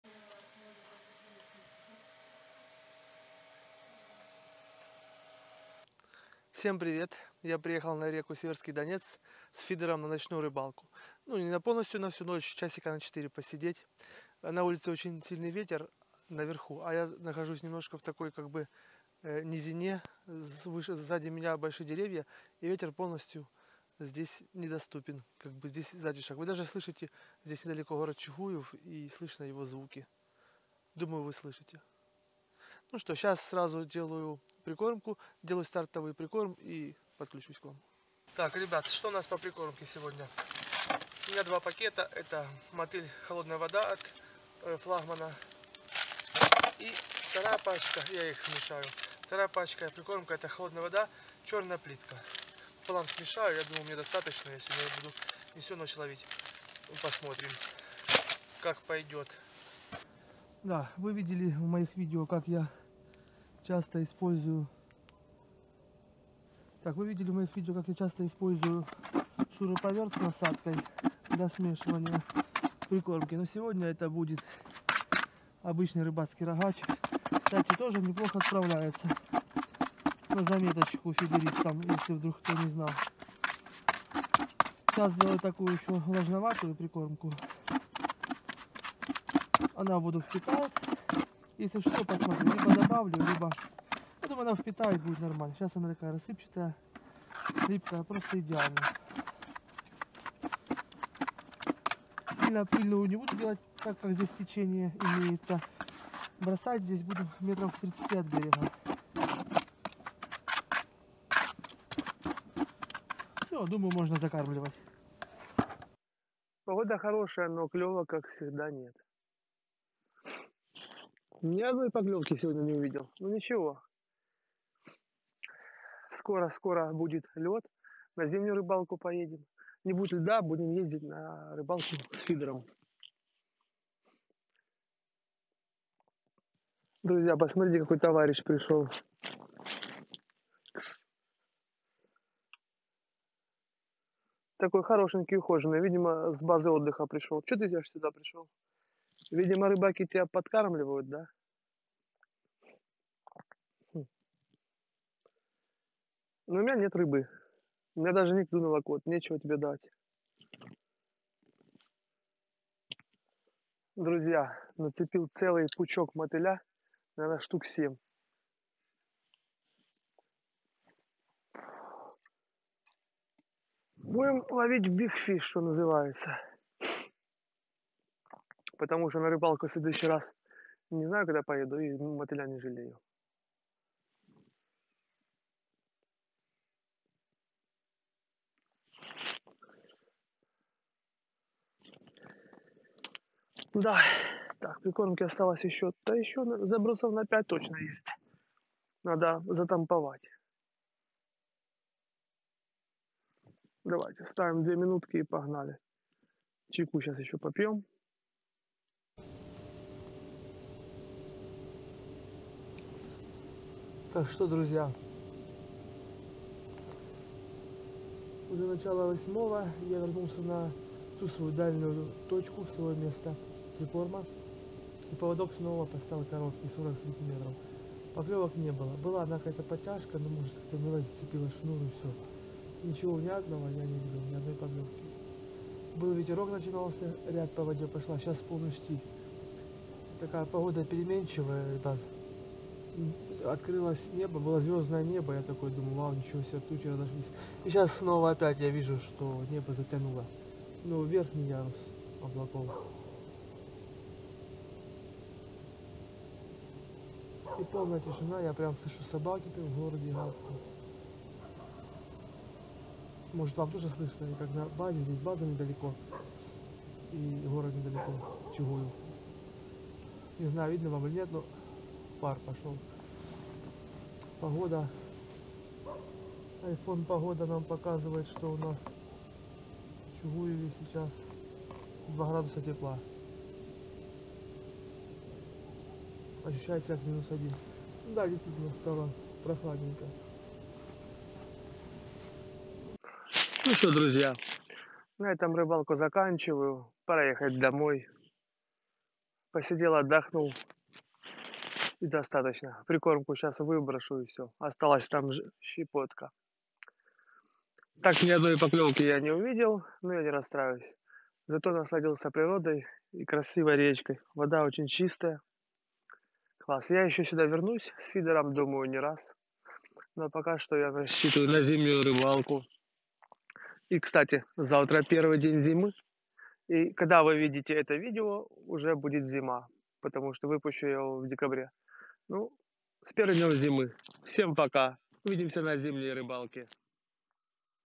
Ловля на ФИДЕР в последний день осени
Приехал на реку Северский Донец посидеть с фидером.
В этом видео я проводил сьемку на две камеры для теста потому что свою экшн камеру я продал.
Сегодня снимал на  canon 80d  и iphone 7.
Ни одна из камер не может нормально снимать ночью! Но все равно решил попробовать.
Это слышно по звуку, в особенности потому что при сьемке на смартфон я использовал блютуз наушники в качестве микрофона.
Скорее всего больше так делать не стану, звуком не доволен.